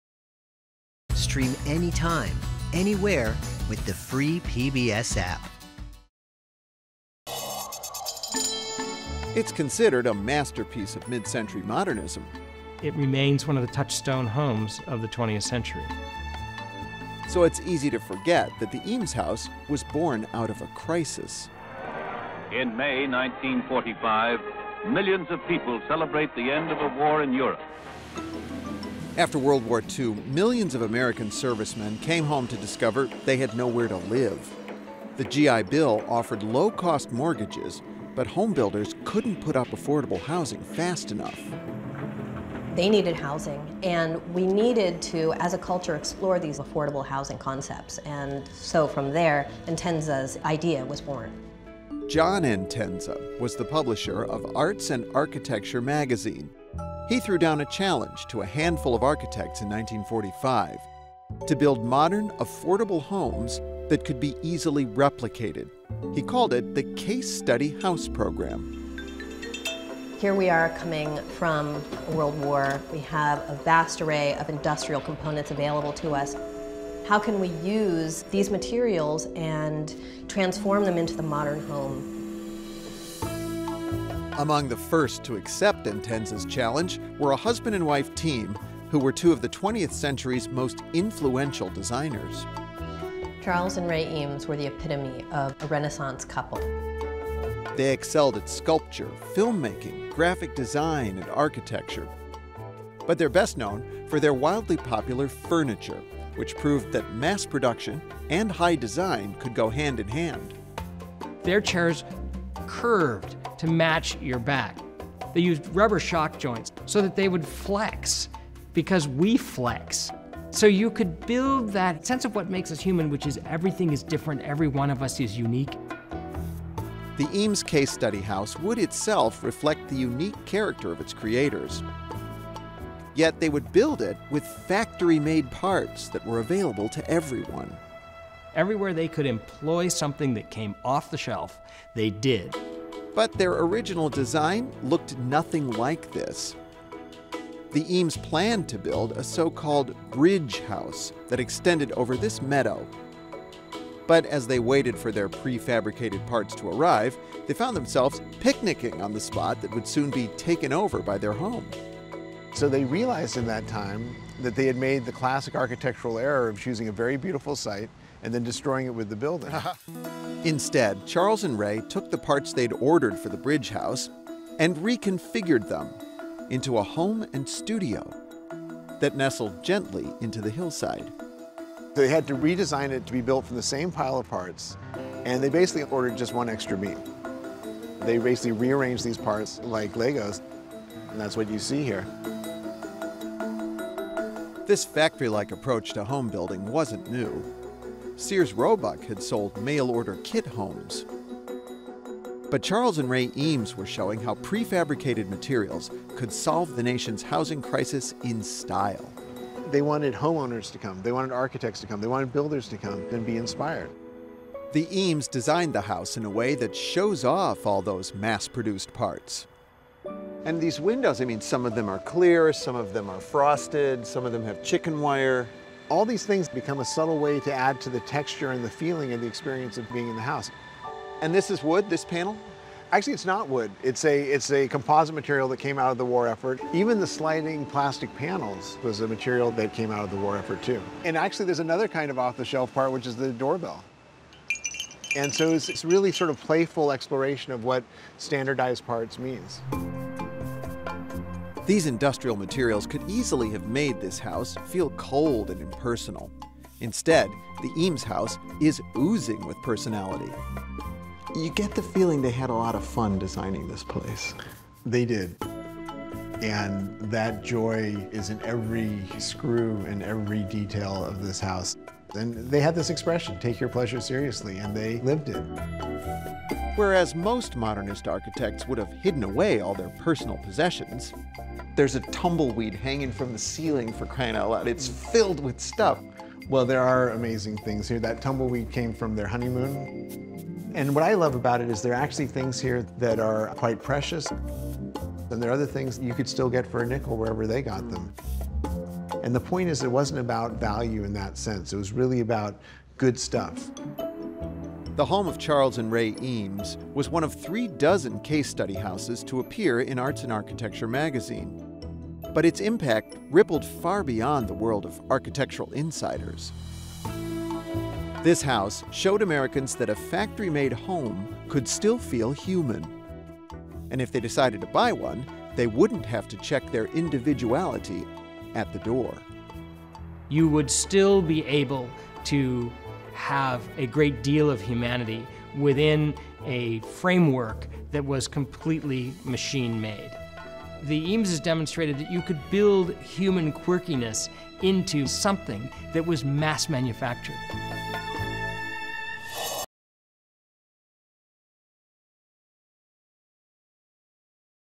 The Midcentury Modern Masterpiece Built Out of Prefabricated Parts
Charles and Ray Eames were influential designers, best-known for their chairs, when they responded to a challenge to build modern, affordable homes that could easily be replicated. Their quirky Eames House used factory-made parts as much as possible, but is still imbued with individuality.